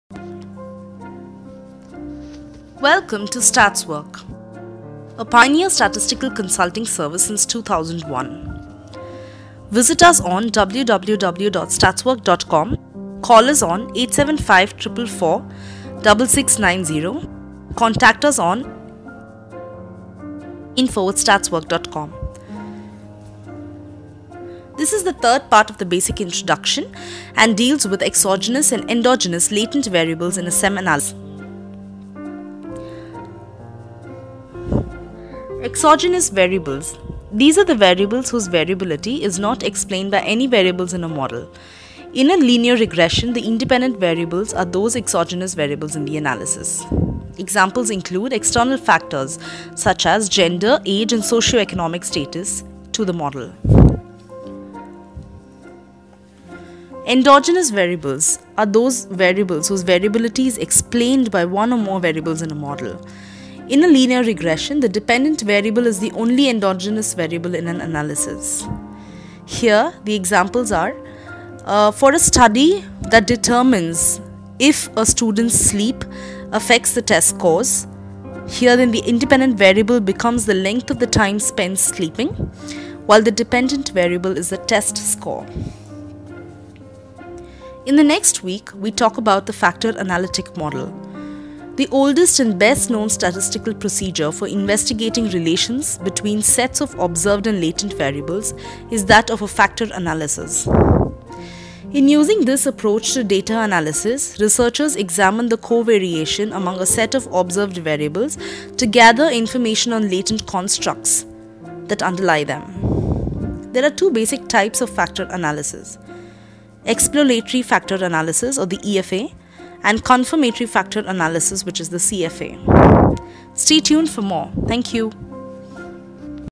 Lecture 3: Exogenous and Endogenous Latent Variables in SEM analysis
In this video we will look at the concept of Exogenous and Endogenous Latent Variables in SEM analysis.